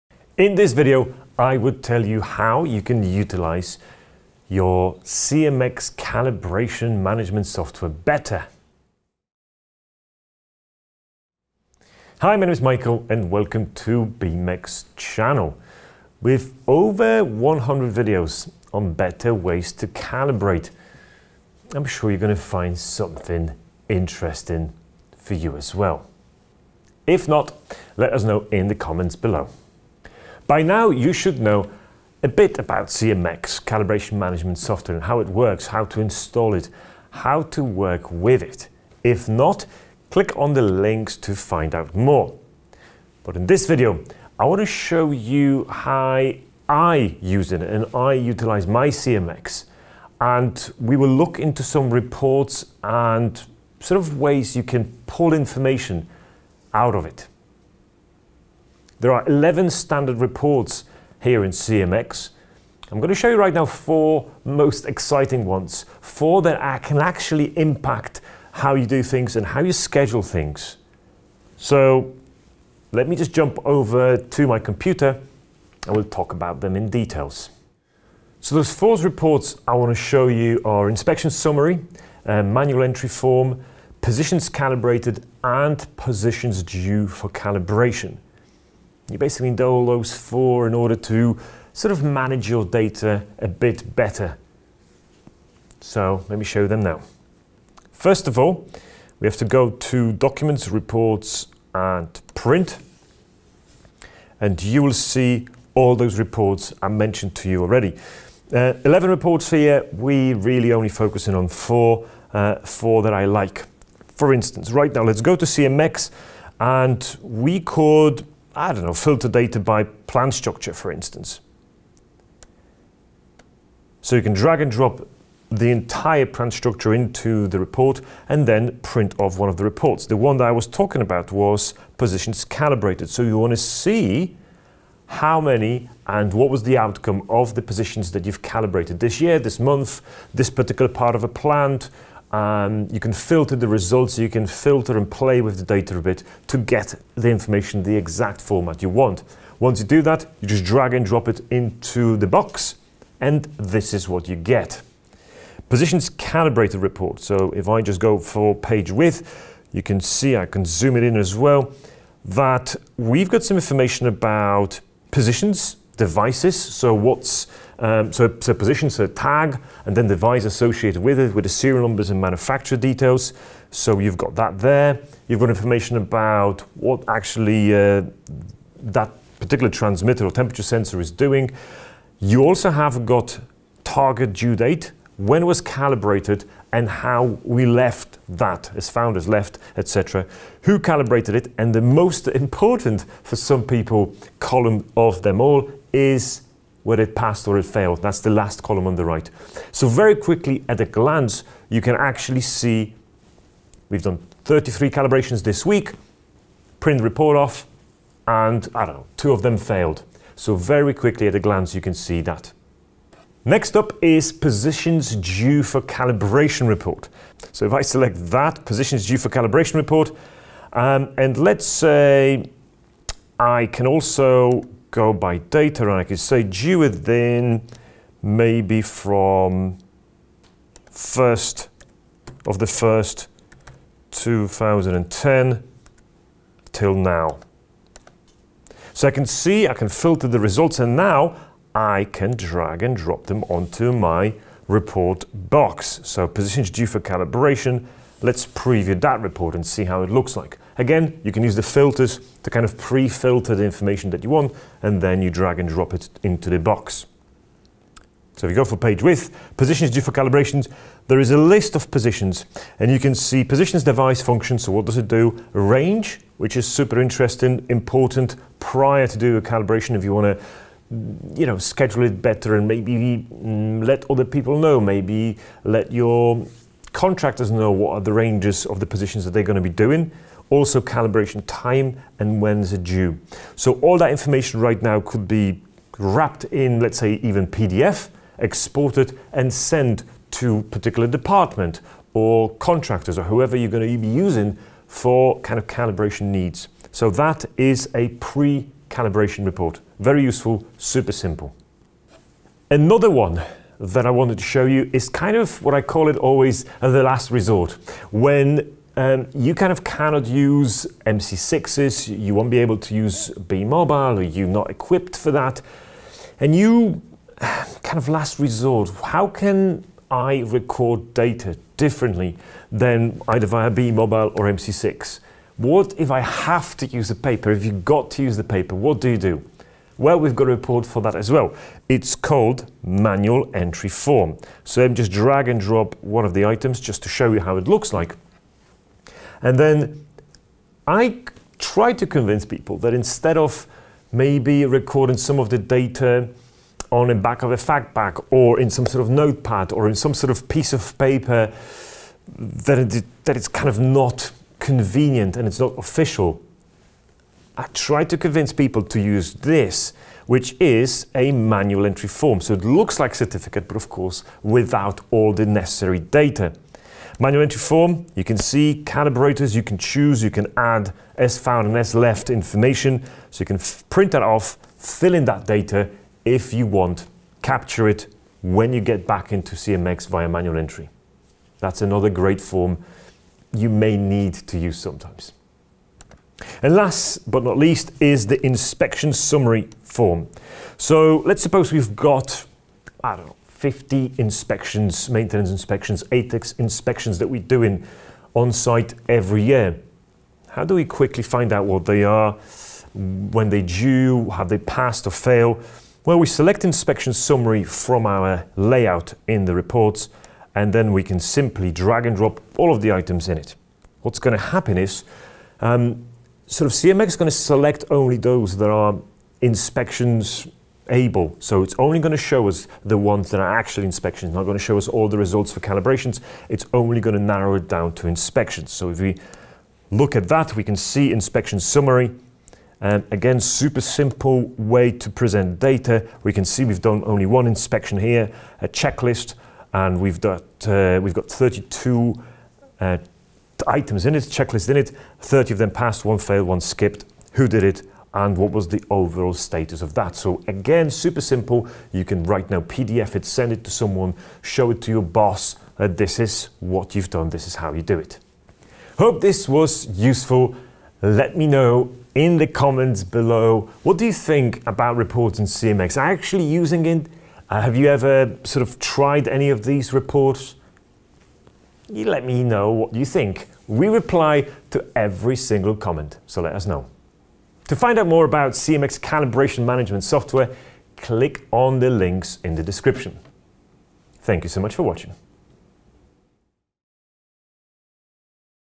Beamex CMX calibration software reports
In this video Mike explains how you can utilize your Beamex CMX calibration software better by using the different reports.

Mike focuses into a few reports: Inspection Summary, Manual Entry Form, Positions Calibrated and Positions Due for Calibration.

Positions Calibrated report show you what instruments have been calibrated during the selected time period. It also shows information on positions and devices, target due date, calibration date, who calibrated it, max found error and if it was a Pass or Fail.

Positions Due for Calibration report show you easily which instruments are due for calibration within the selected time period. Report also shows more information on the instruments. This report is a handy report to list the calibration work that needs to be done.

Manual Entry Form report can be used to help manual entry of calibration results. That can be used if you don’t have documenting calibrators or Beamex bMobile calibration application and have to manually record the calibration results in the field. The form includes fields for all the information that needs to be recorded during the calibration.

Inspection Summary report will list you a summary of all the instruments that are inspection type of instruments. Report lists all inspections that have been done during a given time period and who did them, how many passed and how many failed.